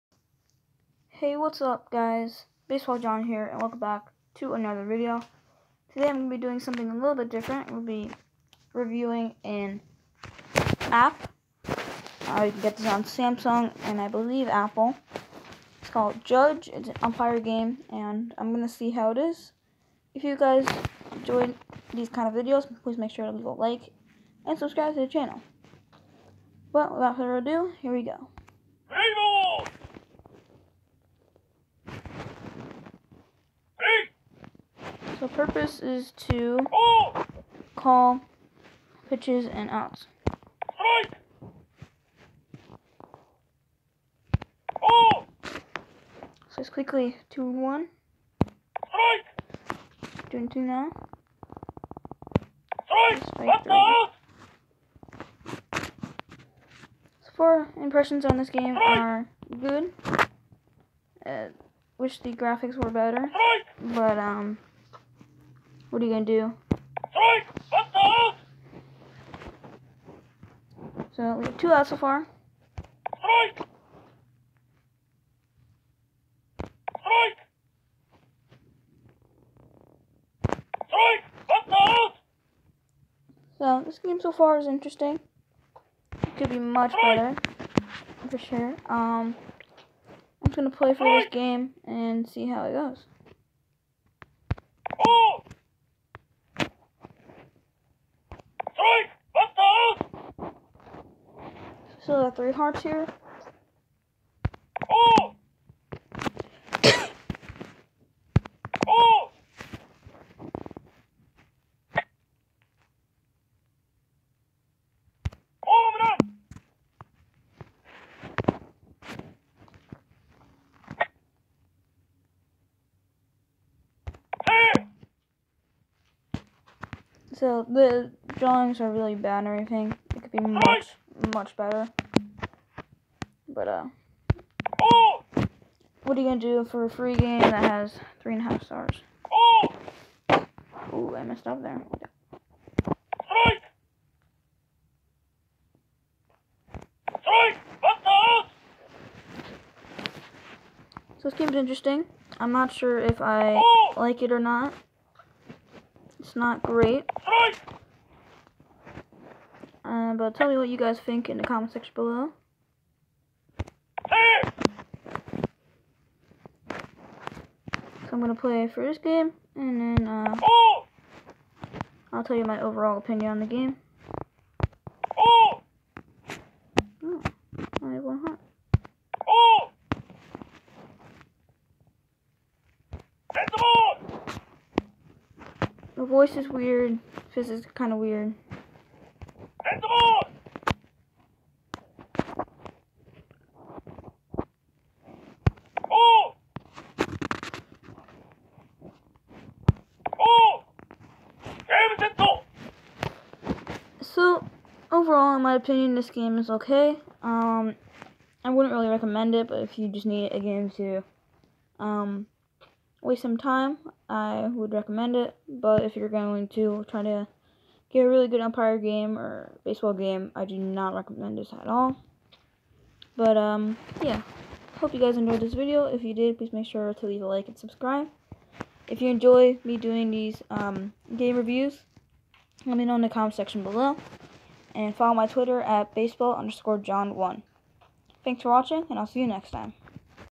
BASEBALL GAME REVIEW #1!!! NEW SERIES!!!
Hey guys, today I started a new series called game review.  In this series, I will review a variety of baseball games and tell you what I think about each game.